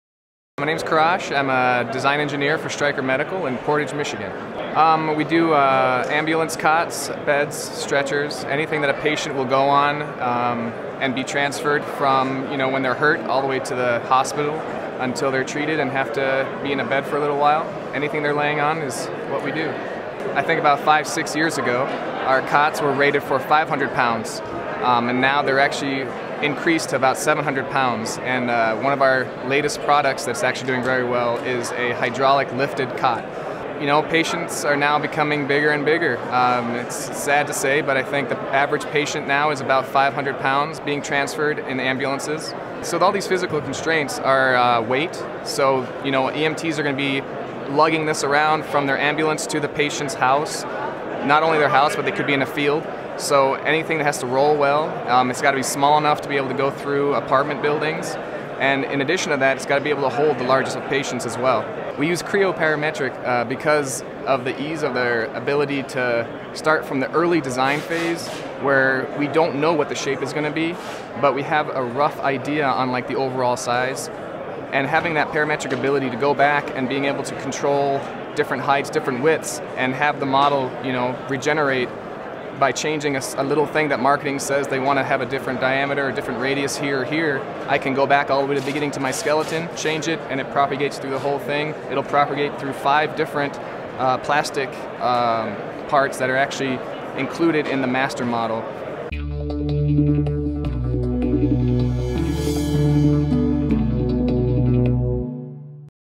Creo Parametric Used in Design of New Hydraulic Lifted Ambulance Cot - PTC
Medical device industry uses Creo in the design of hydraulic lifted cot. Learn how Creo Parametric enabled swift design changes and regeneration throughout the design process.